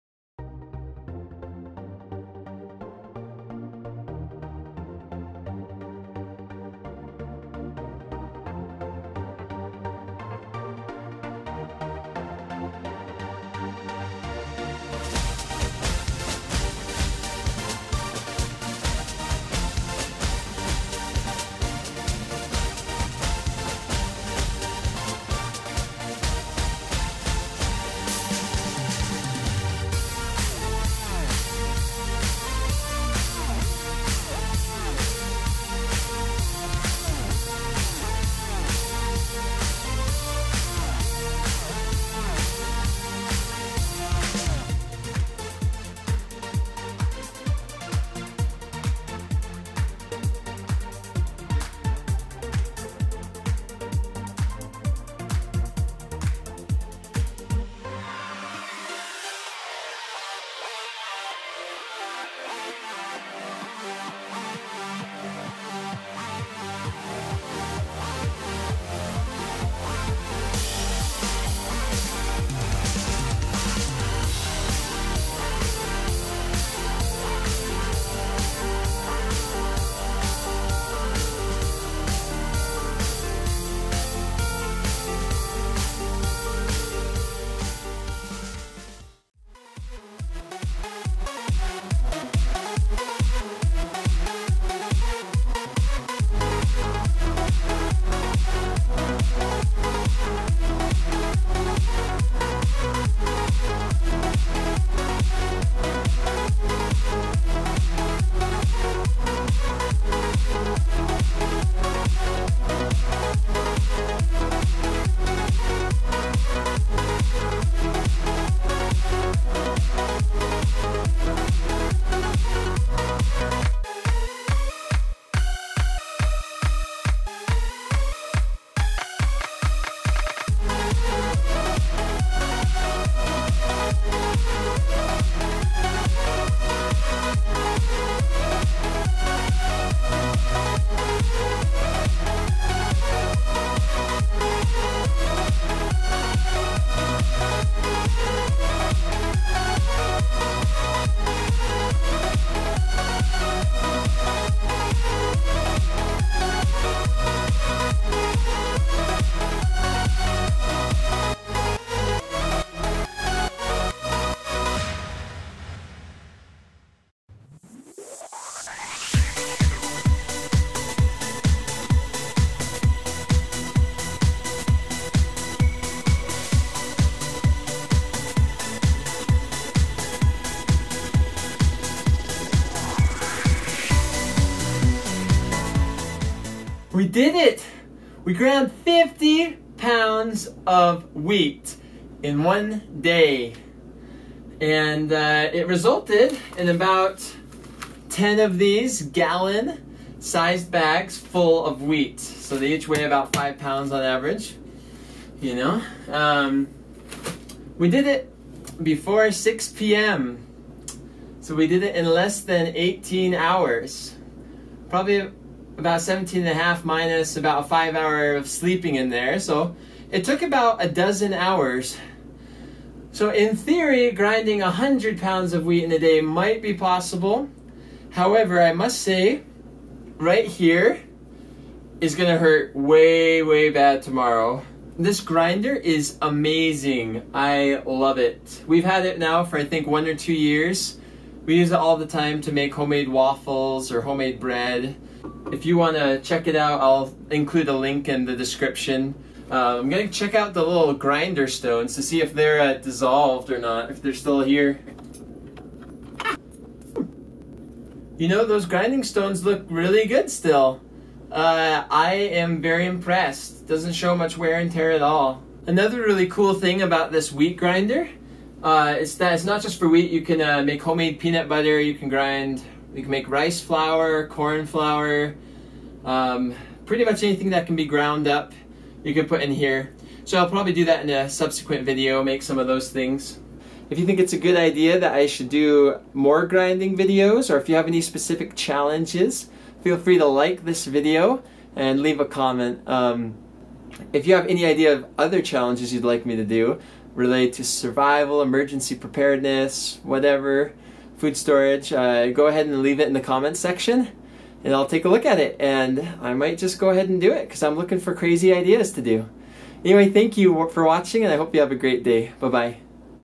MAN VS. WHEAT | Grinding 50LBS Of Wheat (4K Timelapse)
MAN VS. WHEAT | Grinding 50LBS Of Wheat (4K Timelapse). Ultimate test of manliness- grinding 50lbs of flour with a manual wheat grinder.

I've owned the Wondermill Junior Deluxe for the past two years or so and I love it! My wife and I use it all the time. At the end of the video I include a review about the Wondermill Jr.. I thought it'd be fun to see if it's possible to grind 100lbs of flour in one day- I ended up grinding 50lbs in about 12 hours and man I was achy. In the future I may upload tutorials on how to make homemade peanut butter, rice flour or cornmeal.